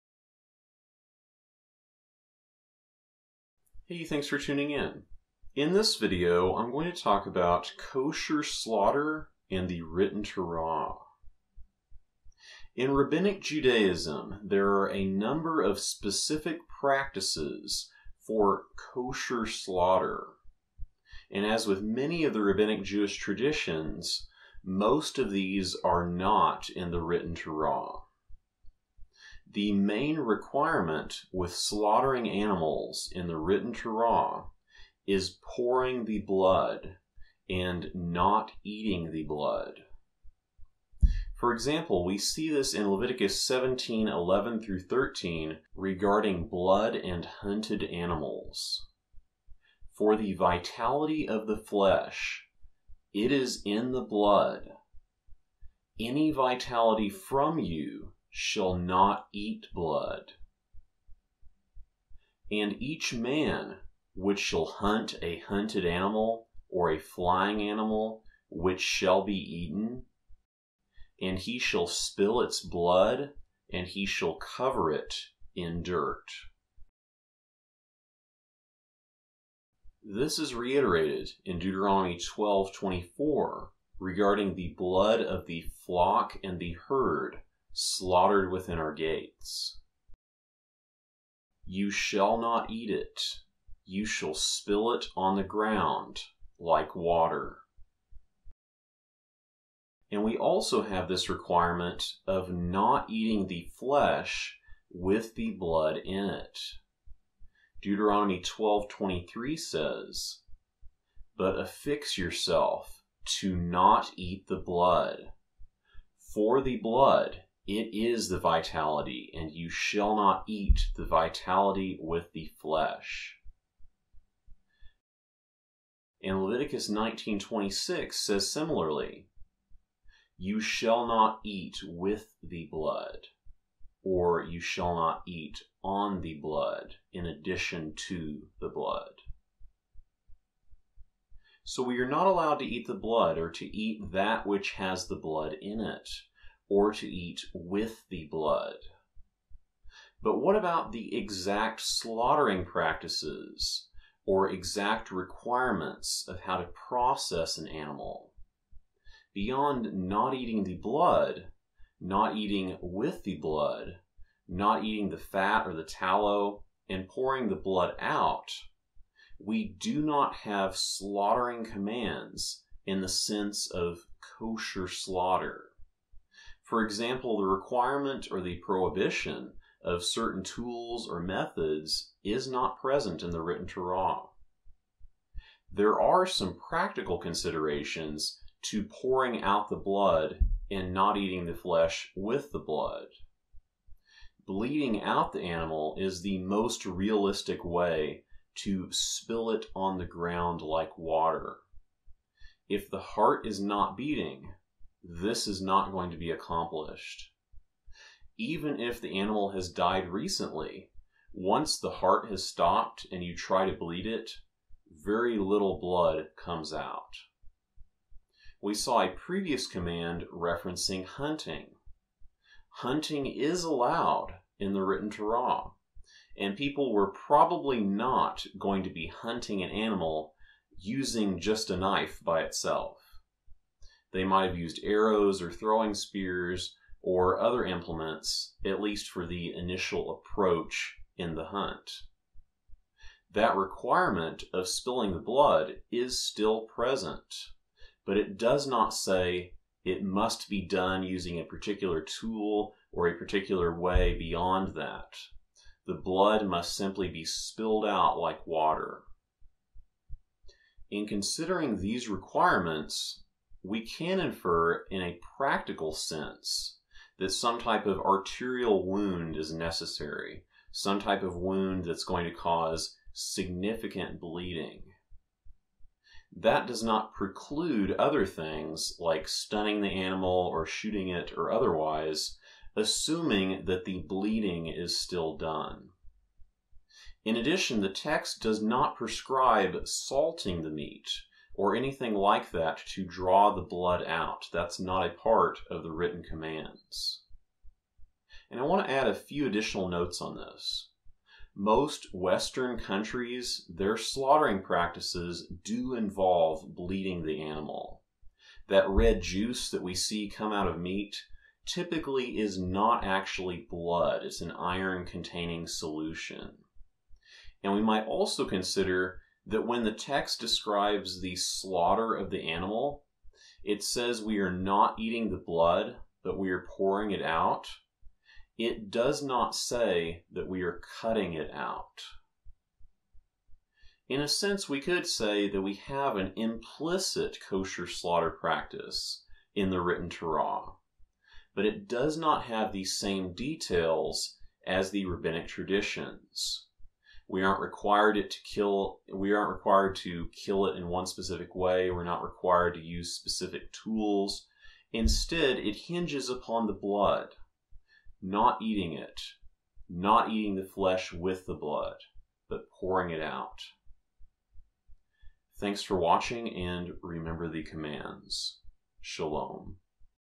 The Kosher Slaughter Implied by the Written Torah
Let's consider the explicit commands and some implicit and practical concerns for slaughtering an animal according to the written Torah standard.

(Thumbnail graphic from public domain)